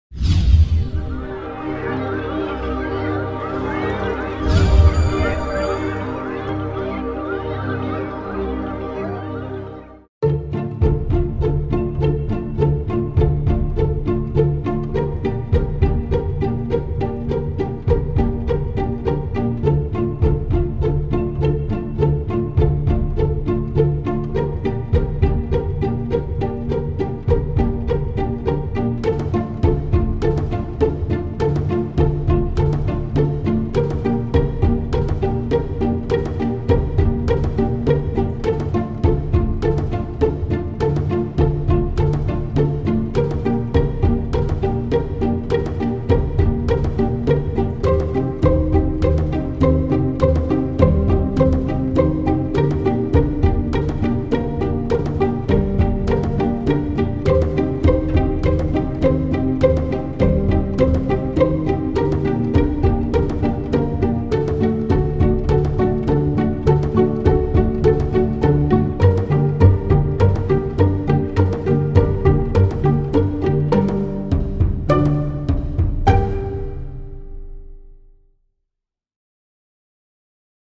« m’dinaEbus » prototype de bus 100% électrique (Short film)
- Un projet pilote entièrement développé dans les ateliers de l’entreprise et par ses compétences internes
- Il s’agit de la conversion de la motorisation thermique d’un bus urbain vers un mode électrique autonome à zéro émission de CO2
- Une solution économique 100% Marocaine qui permet de baisser les coûts du transport urbain